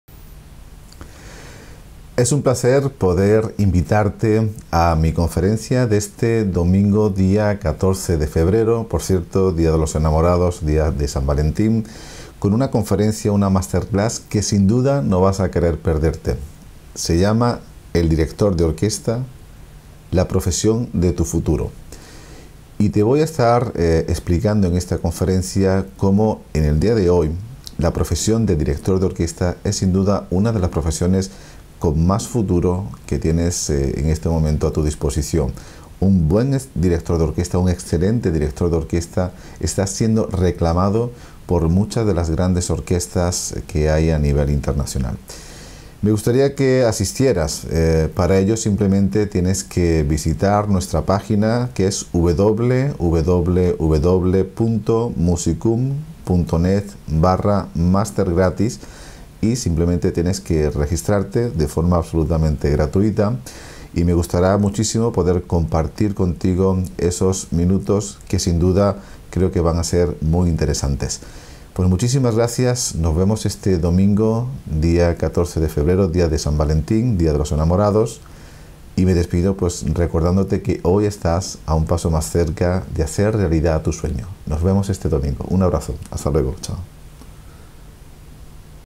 Invitación Conferencia del Maestro Navarro Lara | Domingo 14 de Febrero de 2016
Conferencia Gratis Por Internet: "Director de Orquesta - La Profesión de Tu Futuro" a cargo del Maestro Navarro Lara.

Domingo 14 de Febrero a las 20 horas de España